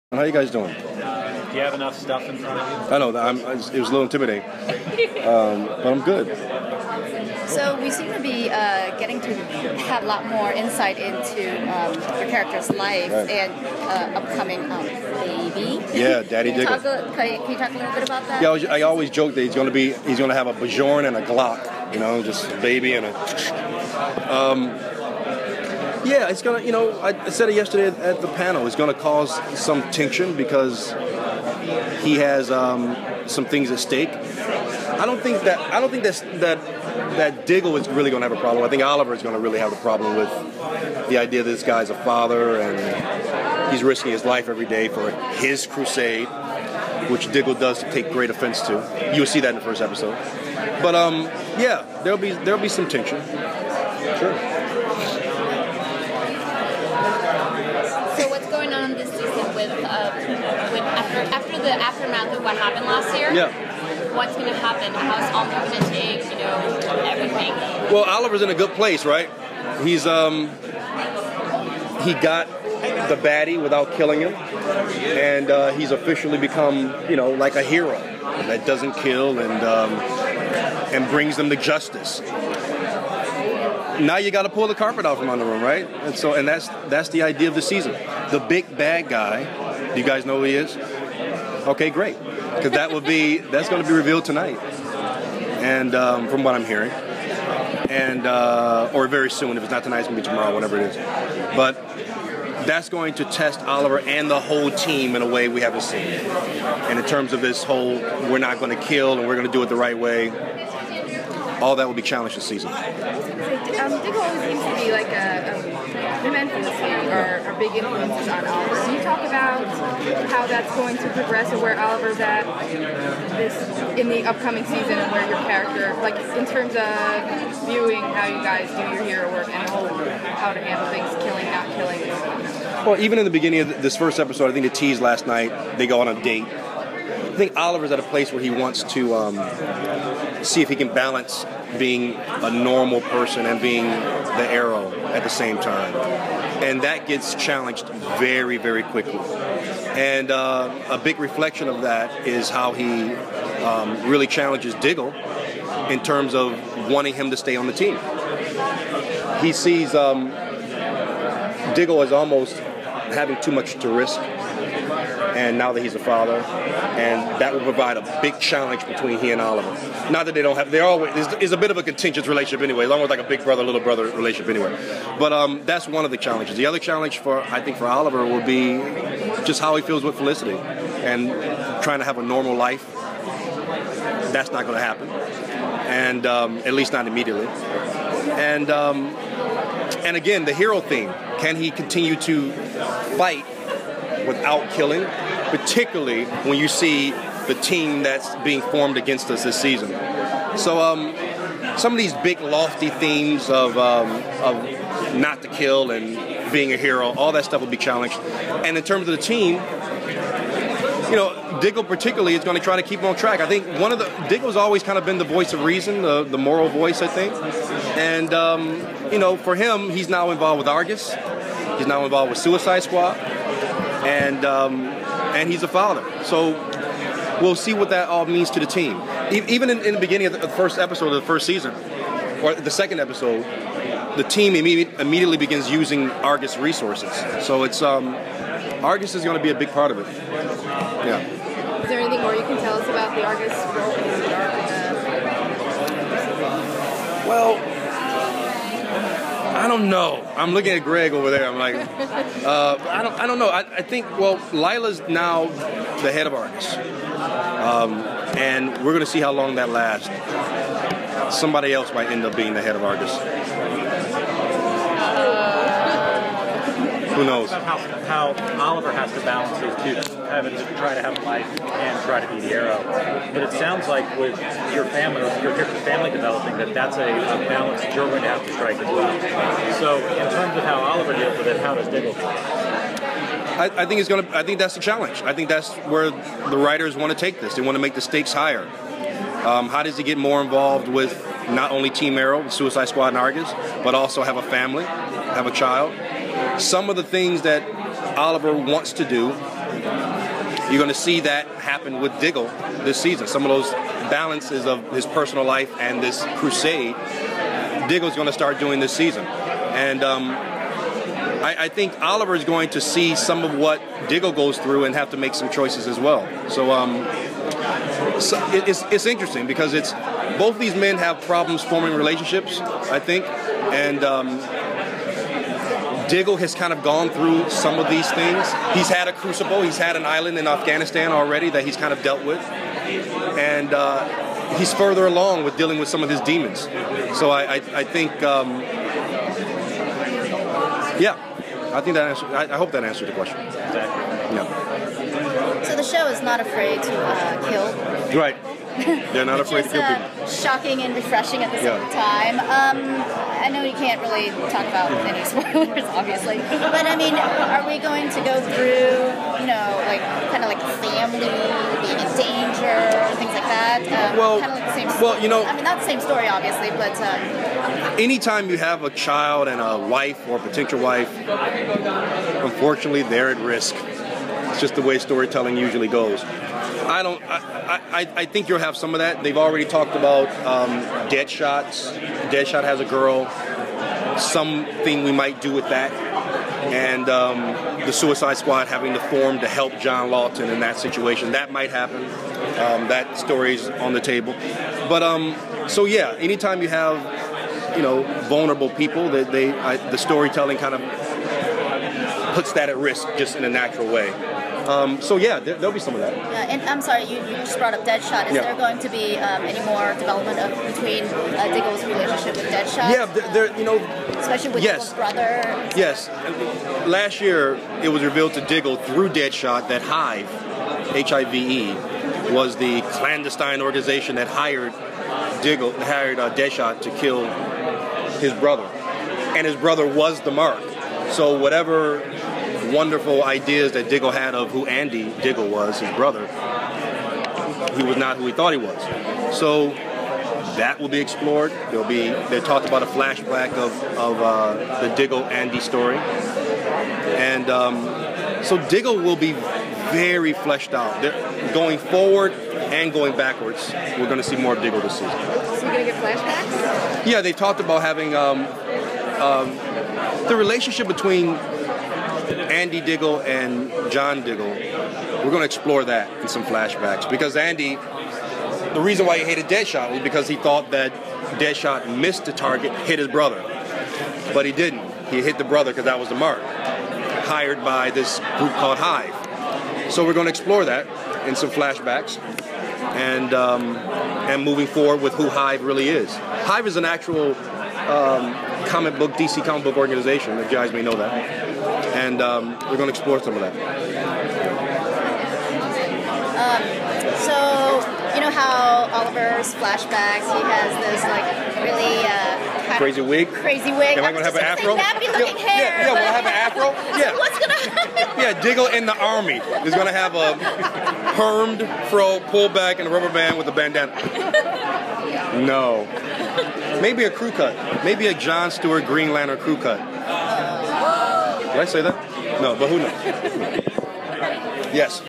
David Ramsey Arrow SDCC 2014
Interview with Arrow's David Ramsey at SDCC 2014